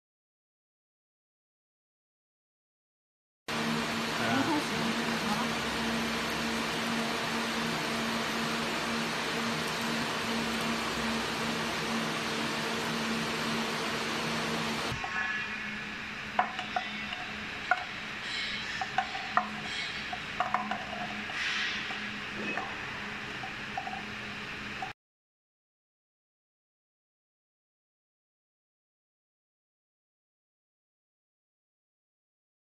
Intelligent Cooling Fan demo
Intelligent cooling fan helps to boost air volume in places like data centers. Easy installation, free of maintenance, energy saving etc